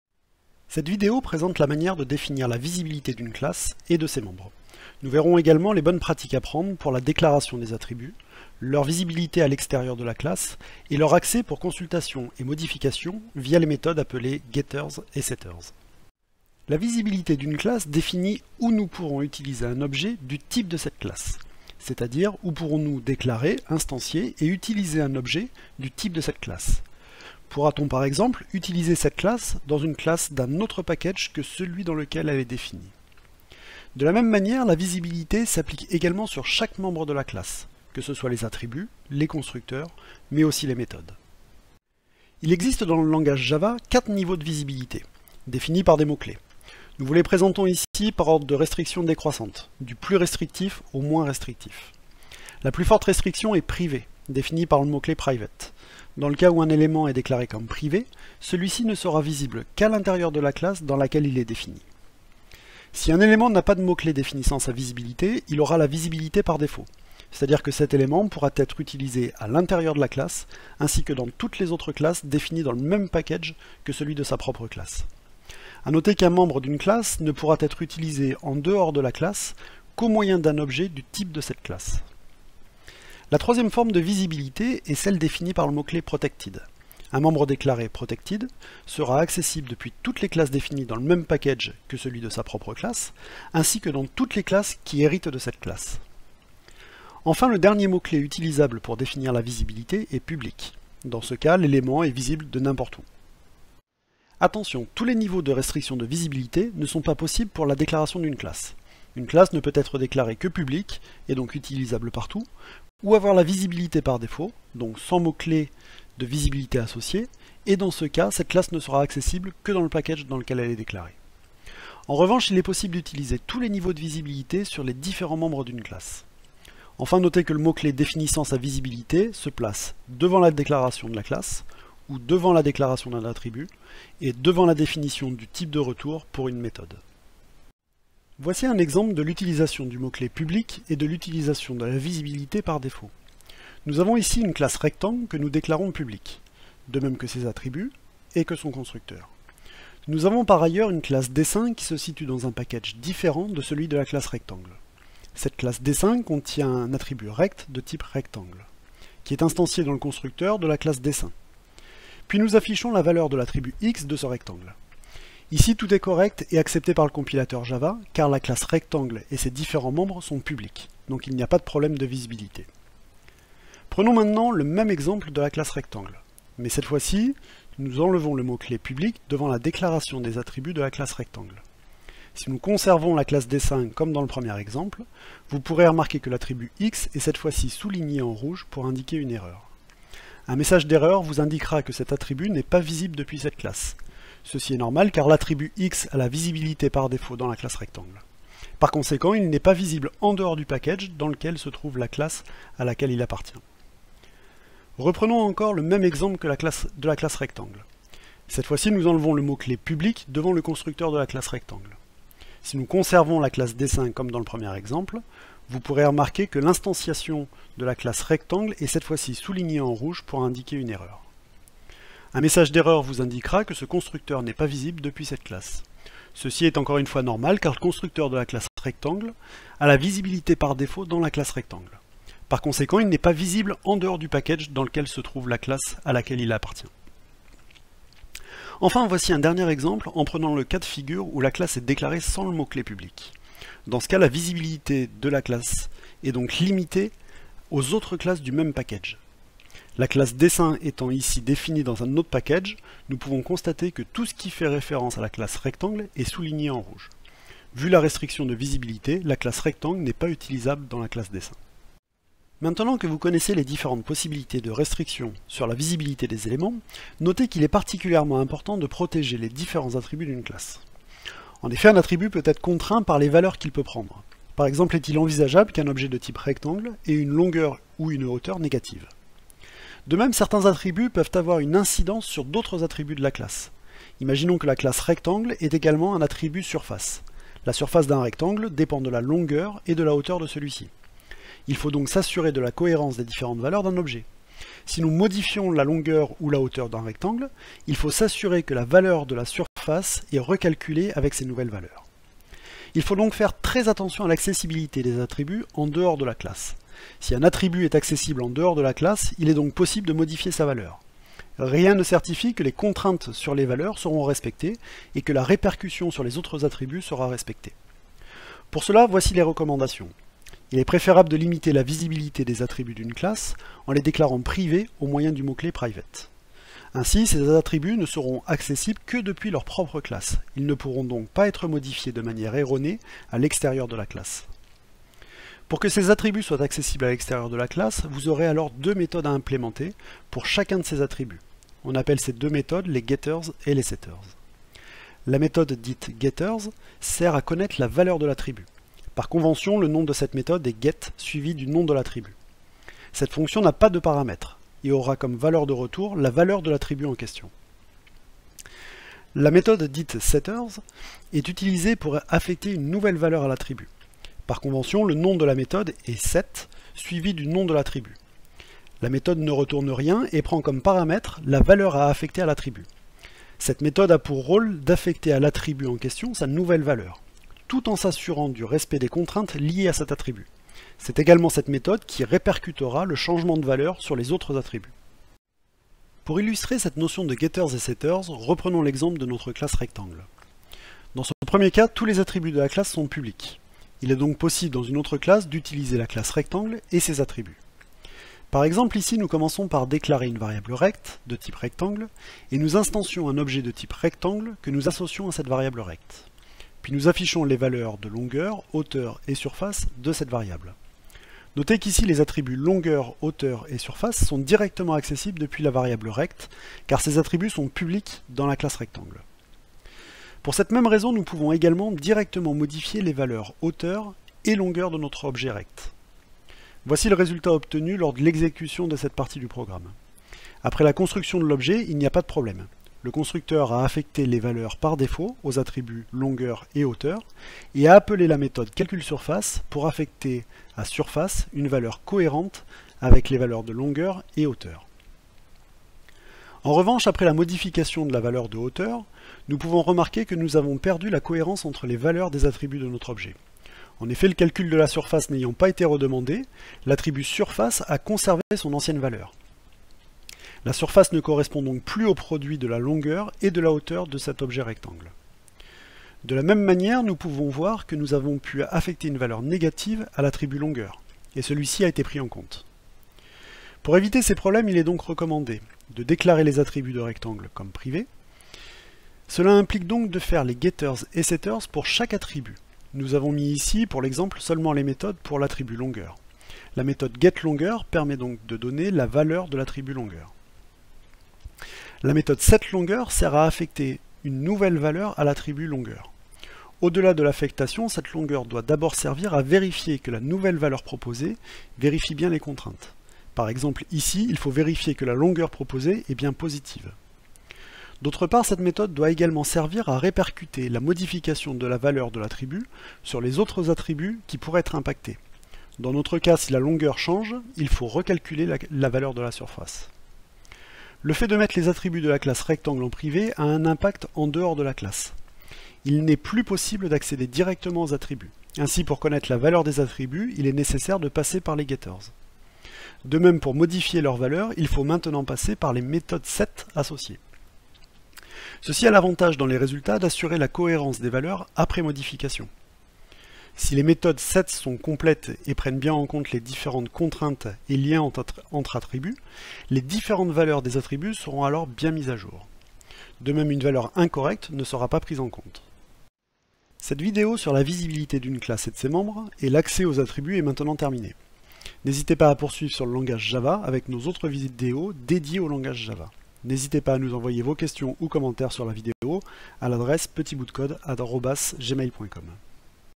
Java - Visibilité d'une classe et de ses attributs
Cette vidéo présente la manière de définir la visibilité d’une classe et de ses membres. Nous verrons également les bonnes pratiques à prendre pour la déclaration des attributs, leur visibilité à extérieur de la classe et leur accès pour consultation et modification via les méthodes appelées getter et setter